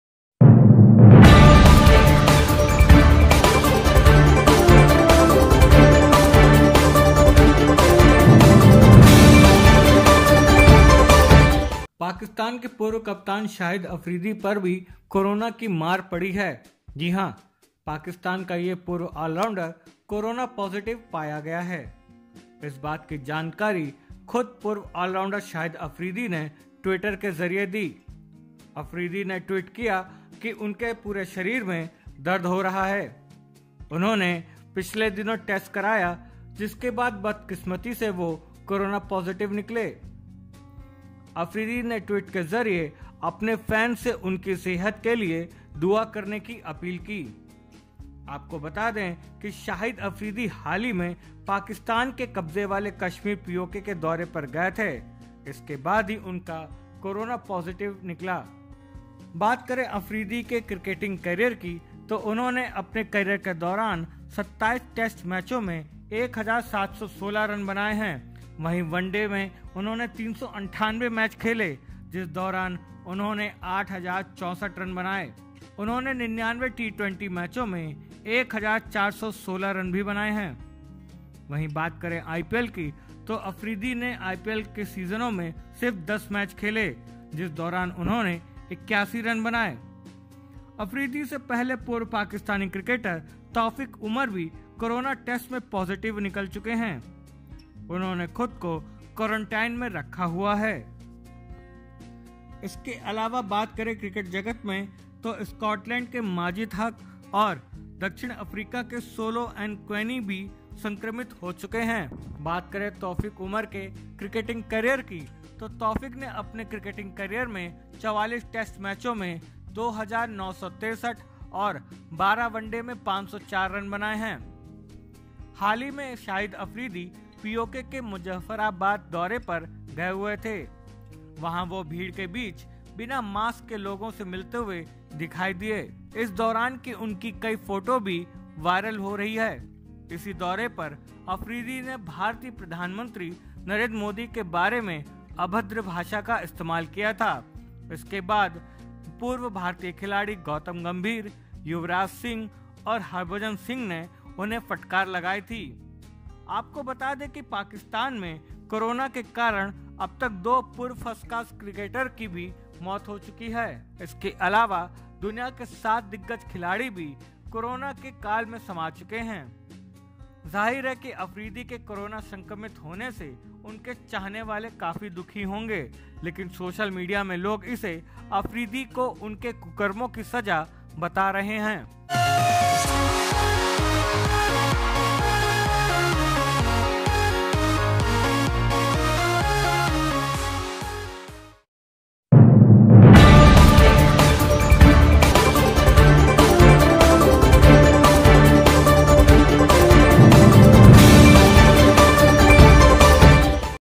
COVID19: अब अफरीदी को हुआ कोरोना Shahid Afridi tests Corona positive
Former Pakistan captain Shahid Afridi said that he has tested positive for Covid-19, becoming the first high-profile cricketer to be infected with the deadly virus.

Another former Pakistan international Taufeeq Umar, had also tested positive for coronavirus but he recovered earlier this month.